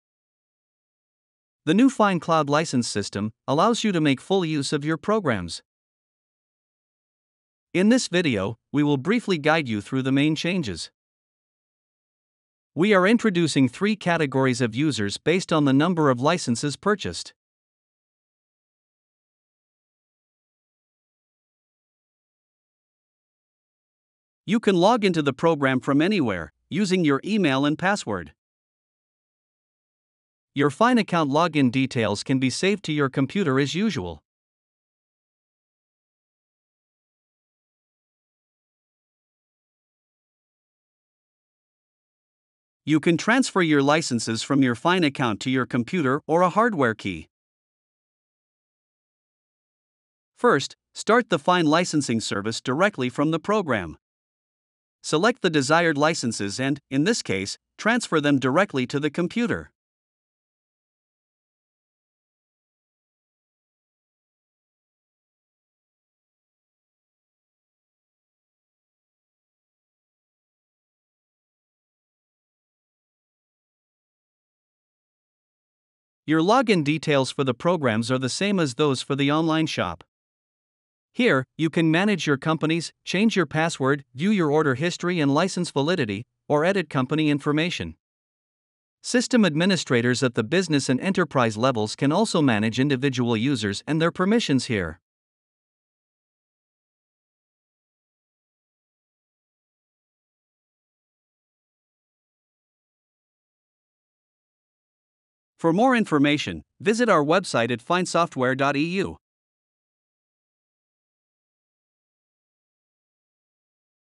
Introducing Fine Cloud License
This video introduces basic work with the new Fine Cloud License system.

GEO5 is a software suite which integrates geological data modelling with the solution of all advanced geotechnical tasks.